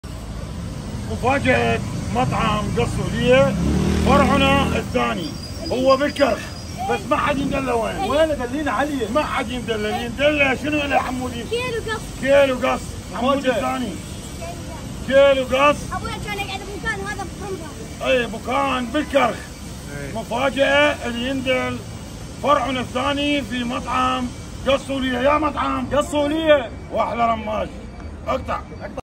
كص وليّة صار بالكرخ 🤩بس وين بالكرخ بقت عليك احزر المكان وين ؟؟١-الدورة!! ٢- السيدية!! ٣- اليرموك!!
كص وليّة صار بالكرخ 🤩
بس وين بالكرخ بقت عليك احزر المكان وين ؟؟
١-الدورة!!      ٢- السيدية!!         ٣- اليرموك!!